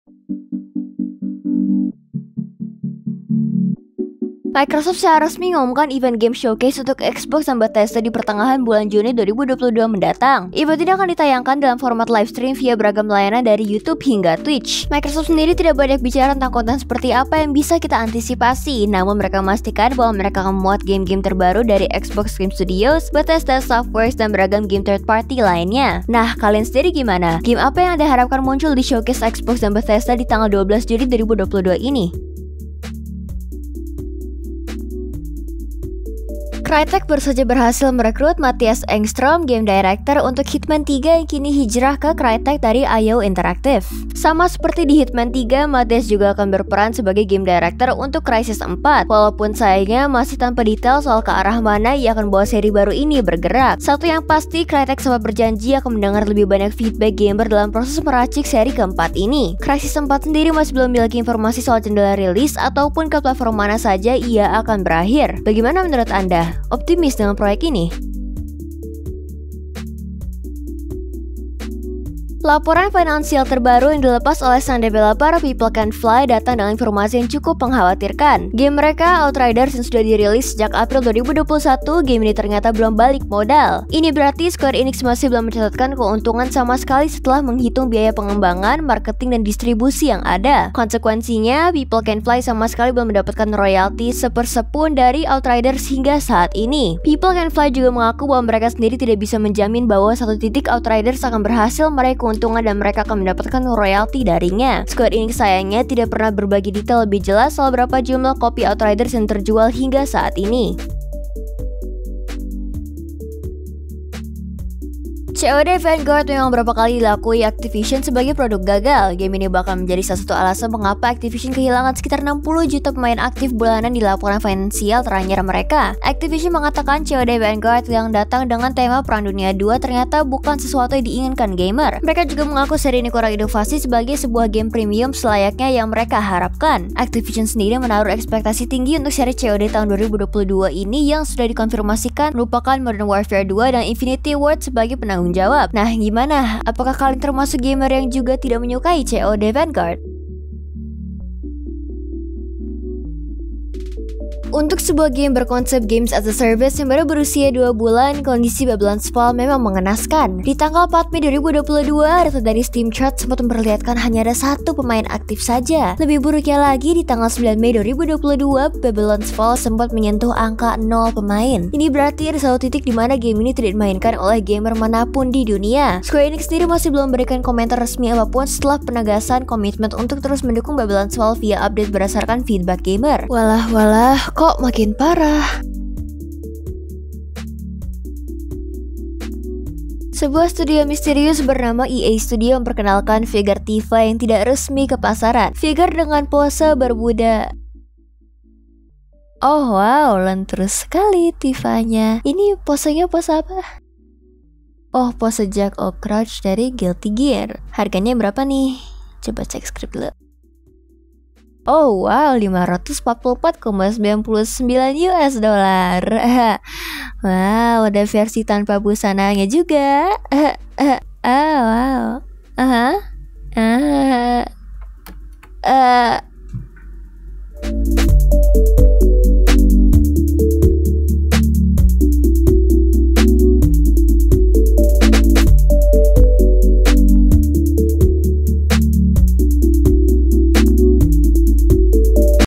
Showcase Game Xbox & Bethesda, COD: Vanguard Kurang Inovasi, Tifa Pose Berbudaya | PlayNews #11, MEI
PlayNews #11 kali ini:
Xbox & Bethesda Akan Gelar Showcase Game di Juni 2022
Game Director Hitman 3 Kini Tangani Crysis 4
Outriders Belum Balik Modal, Developer Belum Dapat Royalti
Activision Akui COD: Vanguard Kurang Inovasi
Babylon’s Fall versi Steam Sentuh Angka NOL Pemain!
Pose “Berbudaya”, Tifa FF7 Dapat Figure Tak Resmi

Timestamp:
00:00 Xbox & Bethesda Akan Gelar Showcase Game di Juni 2022
00:36 Game Director Hitman 3 Kini Tangani Crysis 4
01:12 Outriders Belum Balik Modal, Developer Belum Dapat Royalti
01:55 Activision Akui COD: Vanguard Kurang Inovasi
02:34 Babylon’s Fall versi Steam Sentuh Angka NOL Pemain!
03:10 Pose “Berbudaya”, Tifa FF7 Dapat Figure Tak Resmi